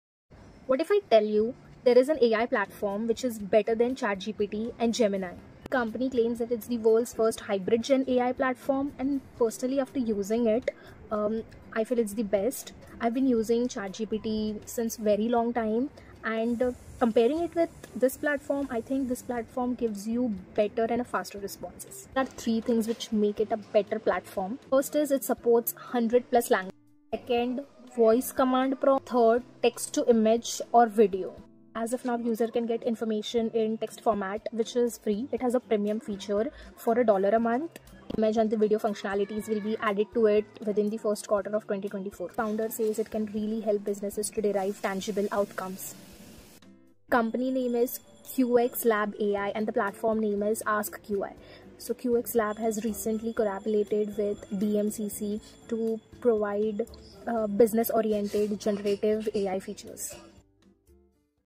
There is Something which is better than ChatGPT 3.5 !
There is an AI better than GPT it's

3 things which makes it best AI platform
   1.Supports 100+ Languages
   2.Voice Command Prompts
   3.Text to image/video
Things I like about AskQx is it's responses are better and faster than  ChatGPT 3.5